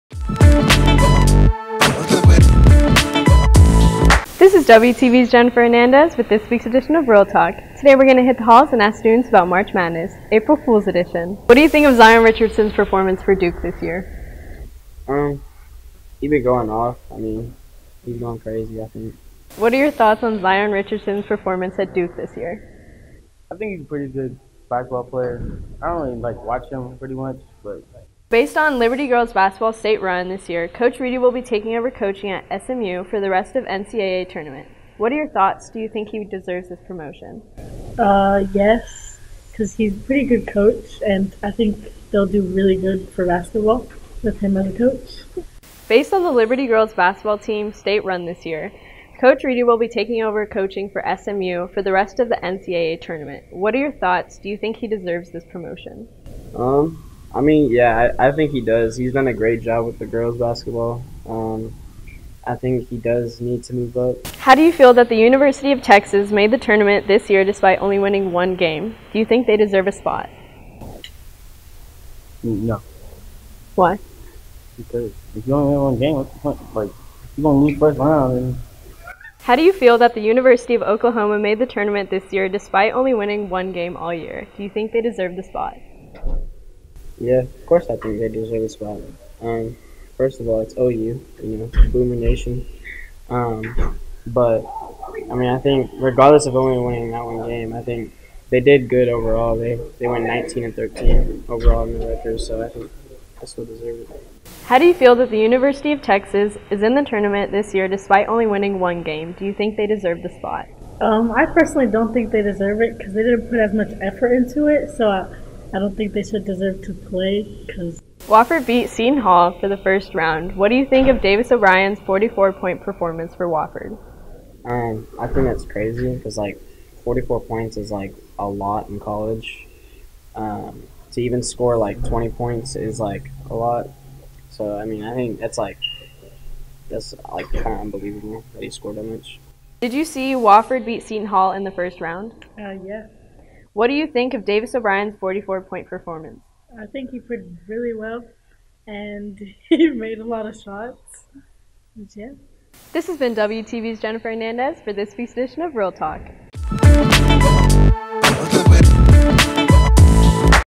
Real Talk: March Madness April Fools Edition
On This week's edition of Real Talk, WTV's Jennifer Hernandez hits the halls to play an April fools joke on students.